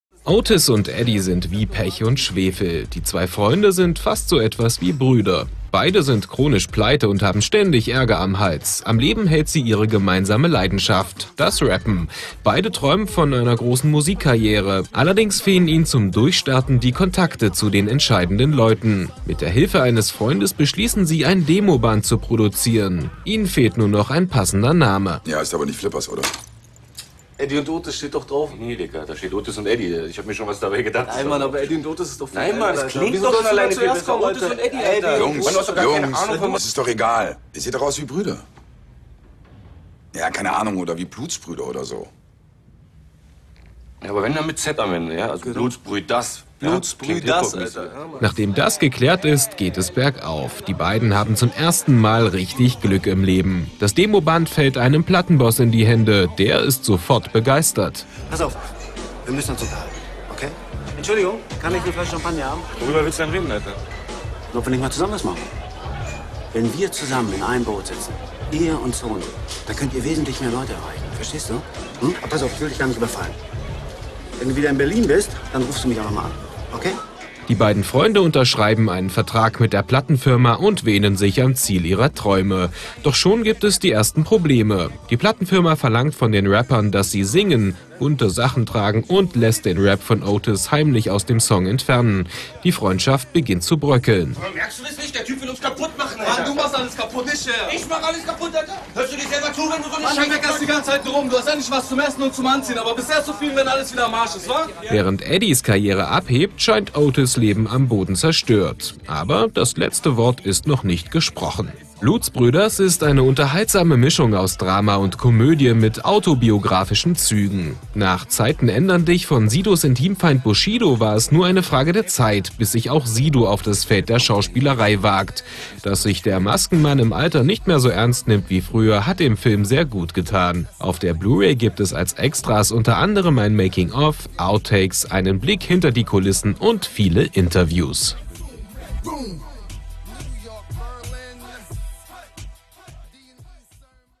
Blu-ray-Filmtipp: Blutzbrüdaz - Video-Podcast zur neuen Blu-ray Disc - Video
Die Jugendlichen lieben ihn, die ältere Generation ist entsetzt. Den Rapper Sido mag man oder man mag ihn nicht. Lange Zeit hatte er sich hinter einer Maske versteckt und nicht ganz jugendfreie Texte gerappt. Über die Jahre ist er ruhiger und solider geworden und er hat es auch von der Musikbühne auf die Kinoleinwand geschafft. In Blutzbrüdaz verarbeitet er die lebhaften Anfangsjahre seiner Musikerkarriere. Der Film ist ab sofort als Blu-ray erhältlich.